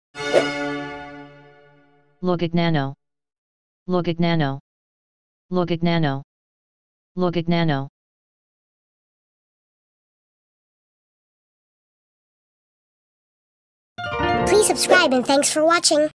Lugagnano (How to Pronounce Cities of the World)💬⭐🌍✅
This video is about "Lugagnano". This video series is something special. We're covering the pronunciation of all the cities of the world.